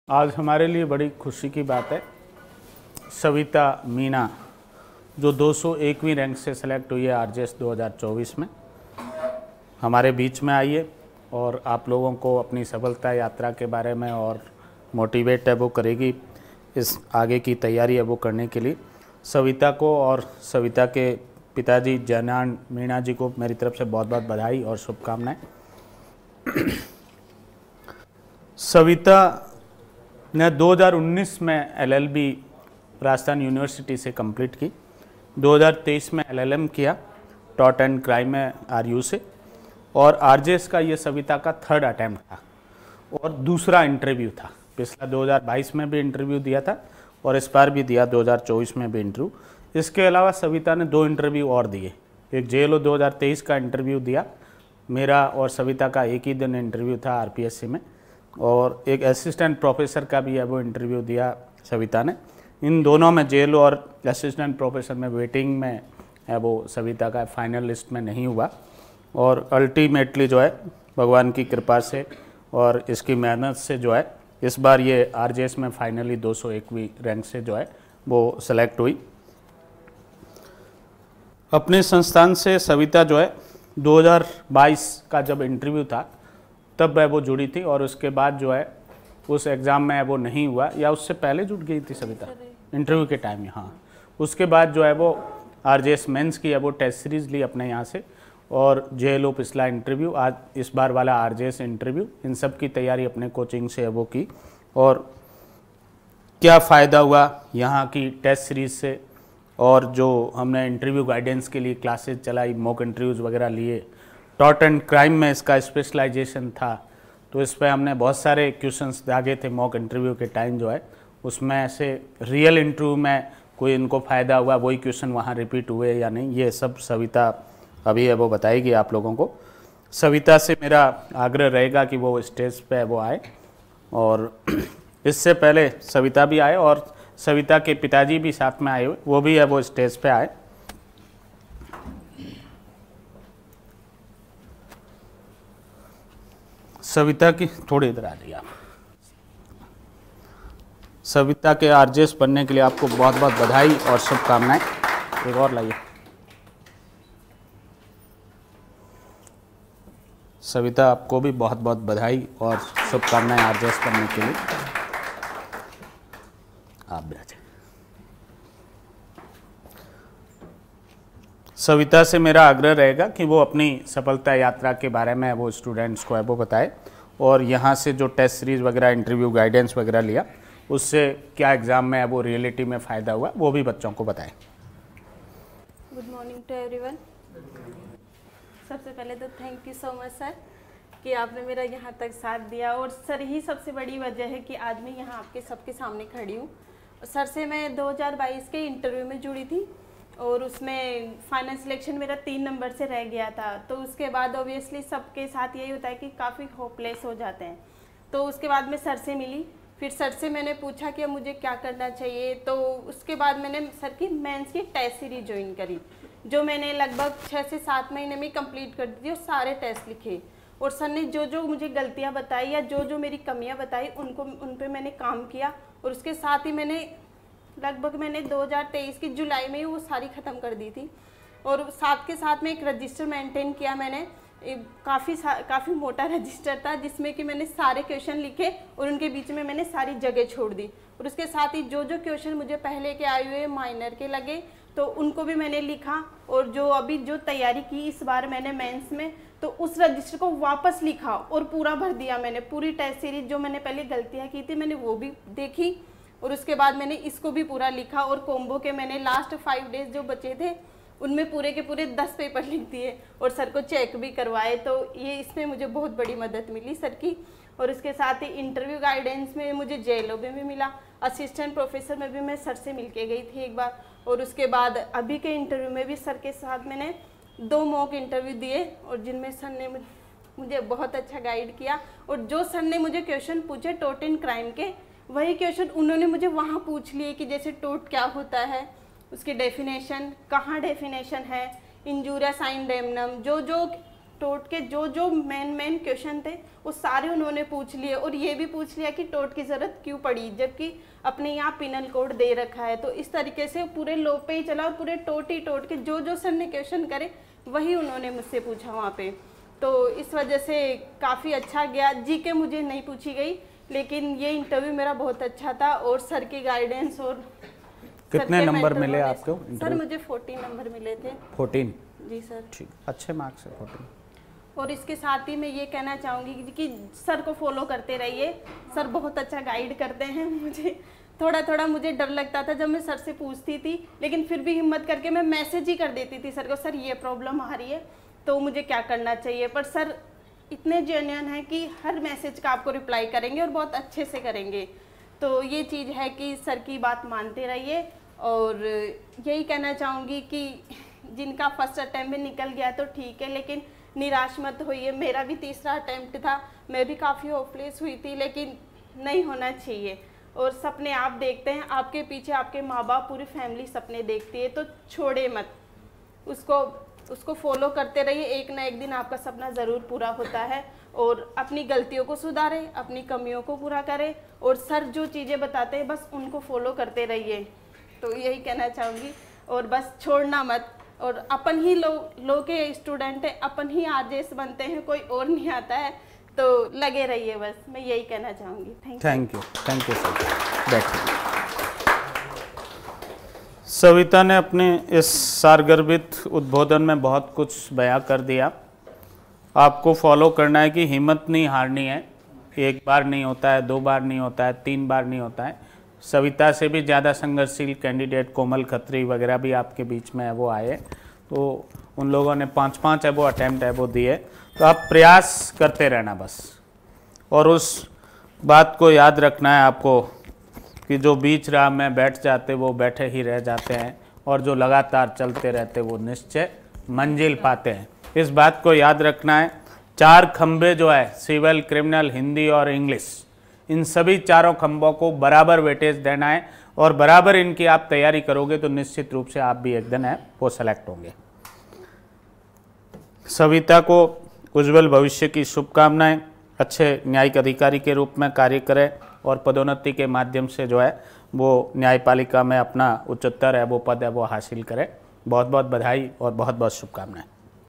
Success story of RJS SAVITA MEENA (Rank-201) ||Strategy For Rajasthan Judicial Exam|| KDALC
SAVITA MEENA has got 201 rank in RJS-2024. We extend our heartiest congratulations to SAVITA MEENA on this great achievement and are gratified to have been a part of her journey to success.
Hope this video helps you in your preparation and we look forward to your feedback in the comments section – don’t hesitate to ask!